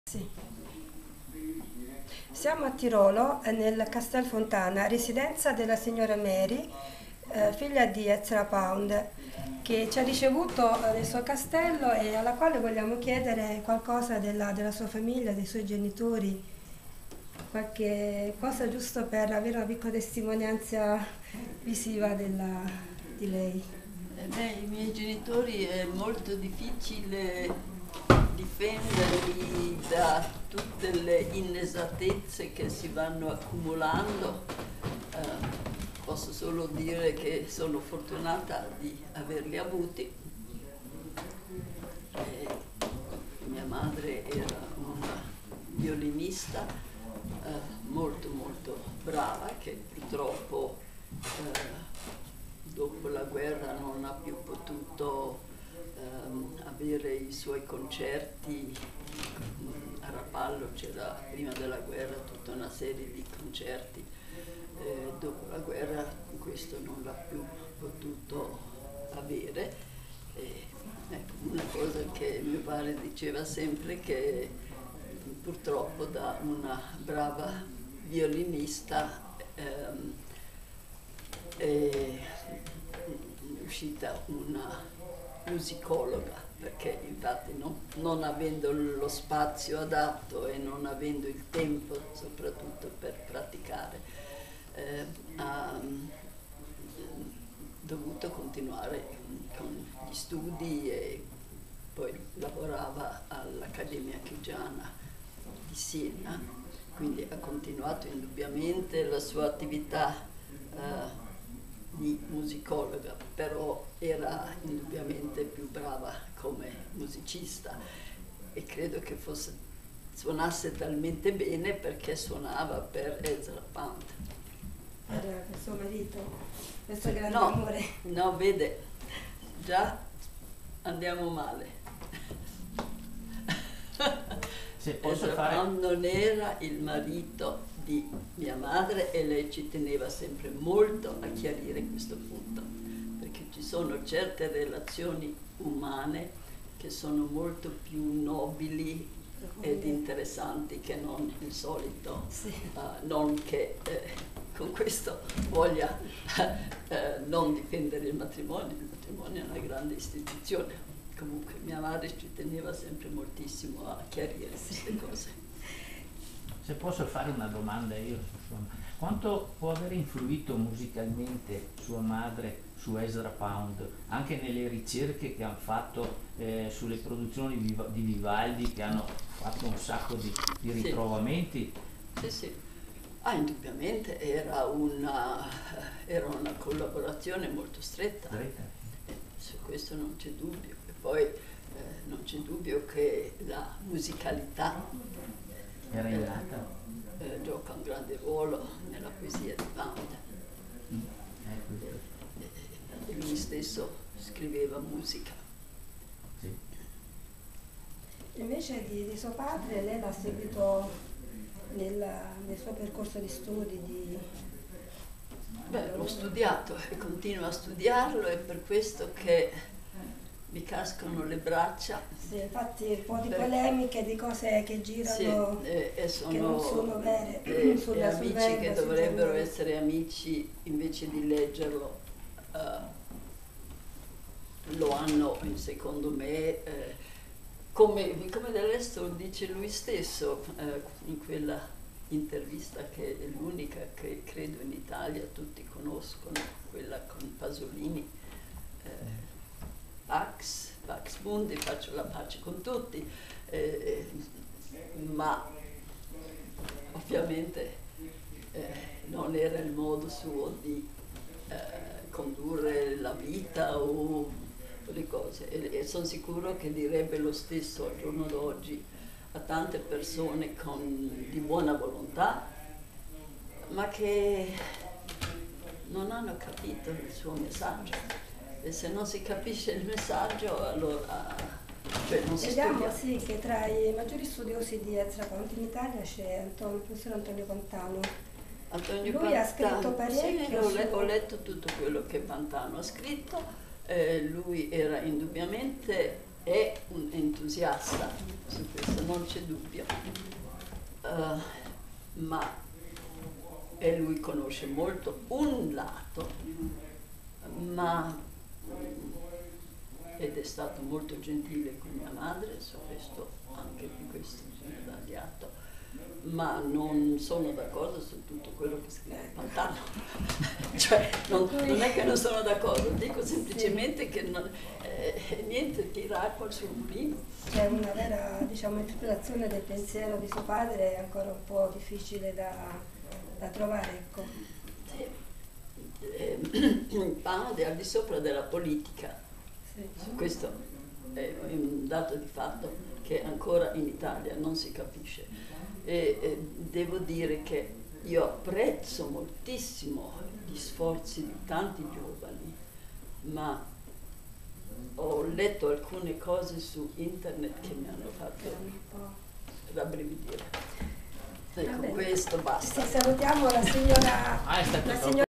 Giovanna Canzano incontra Mary de Rachewiltz 29.8.2010.MOV
Mary  de Rachewiltz figlia di Ezra Pound e Olga Rudge
Castel Fontana (ted.: Brunnenburg)  presso Tirolo, in val Venosta (BZ).